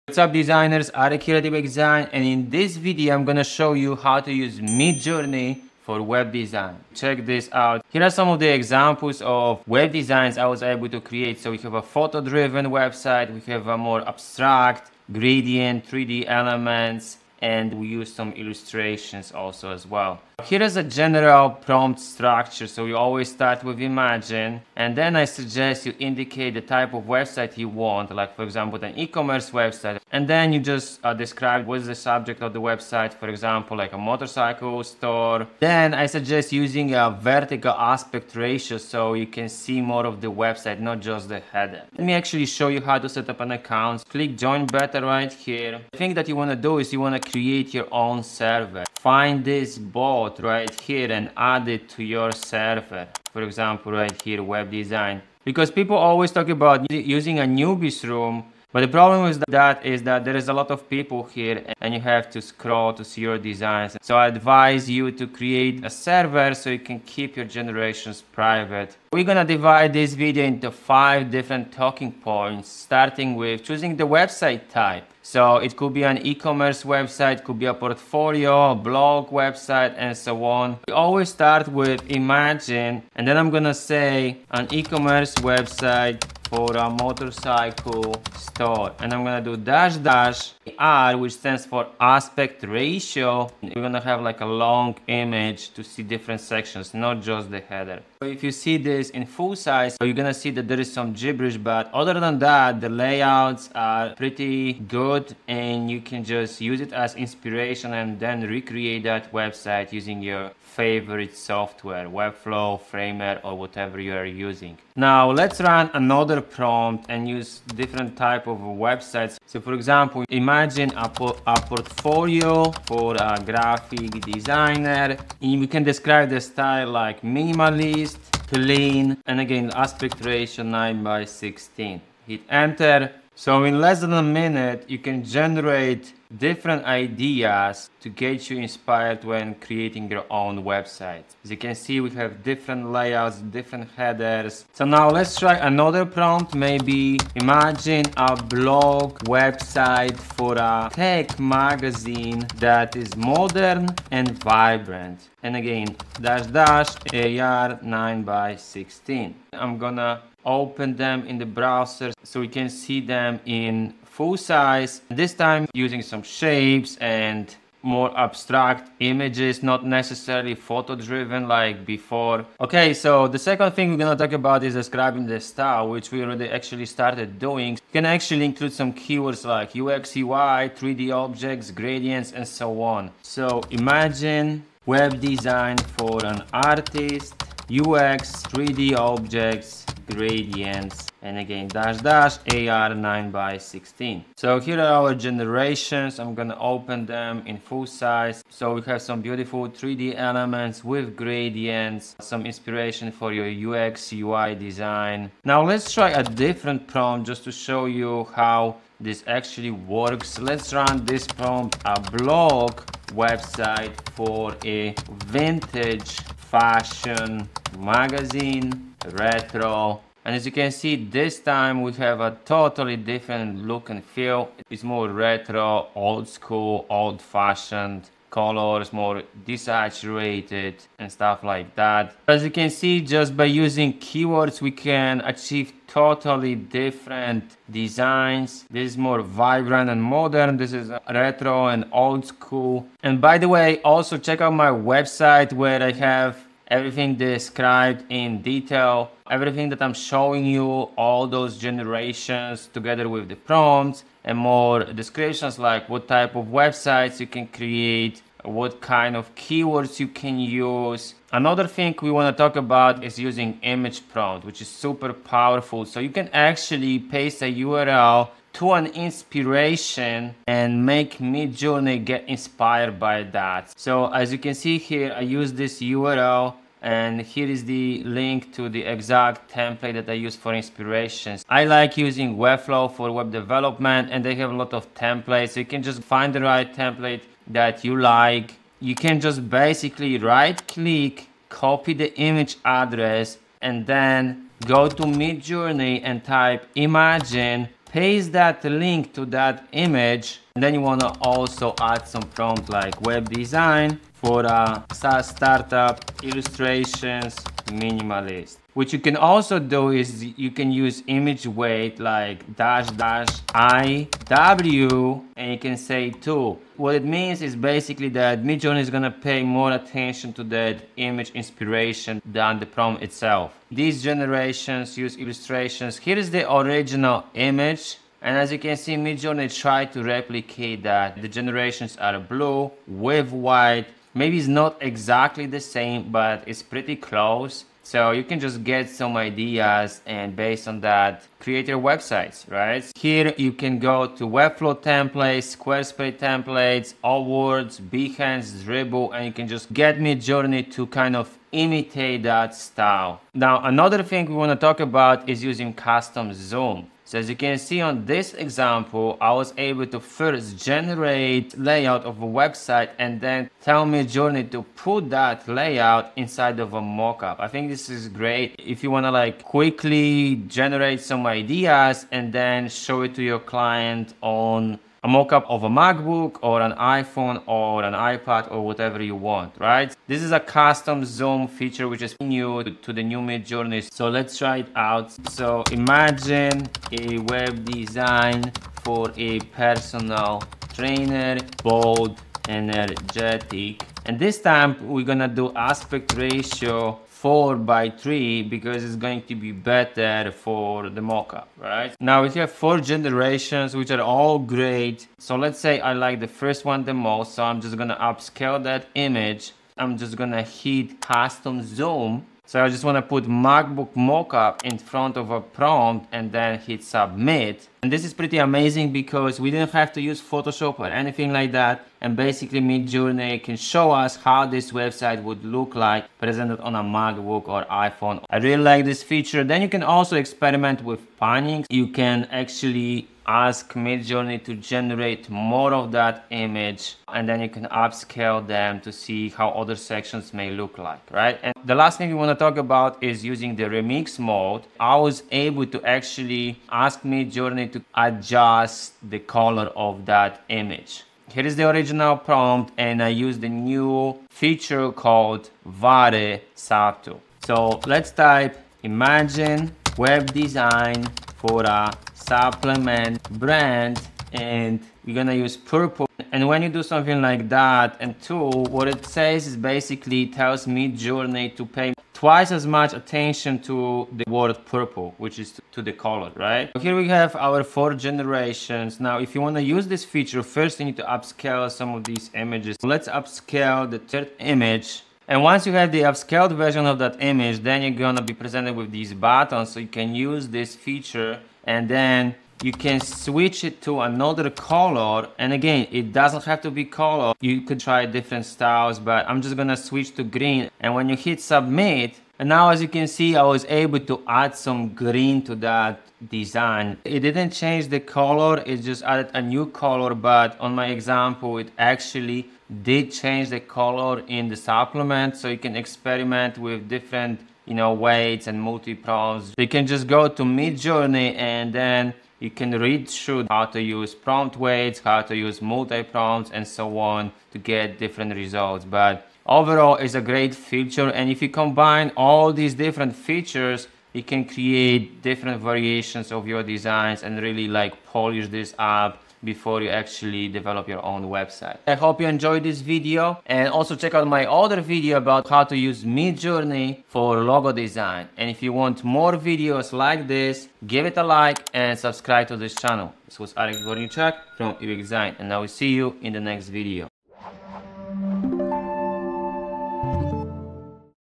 How To Use Midjourney For Web Design (Best Prompts)
Are you looking for a way to quickly generate web design ideas?

In this video, I'll guide you through the process of using Mid Journey, a bot on Discord that can help you create website concepts.

CONTENTS:
00:20 Midjourney Prompt for Web Design
00:41 How to sign up for Midjourney
01:12 Choose website type
02:52 Describe the style
04:21 Use image prompt
05:53 Use custom zoom
07:21 Use remix mode

Key Takeaways:
— Choose the type of website you want to design (e.g., e-commerce, portfolio, blog) to guide Midjourney's design generation.
— Use keywords to describe the style you want (e.g., UX, UI, 3D objects, gradients).
— Use image prompts by pasting a URL to an image that inspires you.
— Use the custom zoom feature to place your design inside a mockup (like a MacBook or iPhone).
— Use the remix mode to adjust the color of an image.
— Set up an account and create a private server for working with Midjourney.
— Use a vertical aspect ratio to see more of the website, not just the header.
— Use prompt weights and multi-prompts to emphasize certain elements in your design prompts.
— Use Midjourney for inspiration, then refine the designs using your preferred design software.
— Check out additional resources for more detailed instructions and examples.

As an expert in web design, I believe that sharing this video will be valuable to those who want to create beautiful websites quickly and efficiently.

Prompt example:
/imagine an eCommerce website for a motorcycle store —ar 9:16

Customize your web designs by modifying the prompt—watch my video to see examples.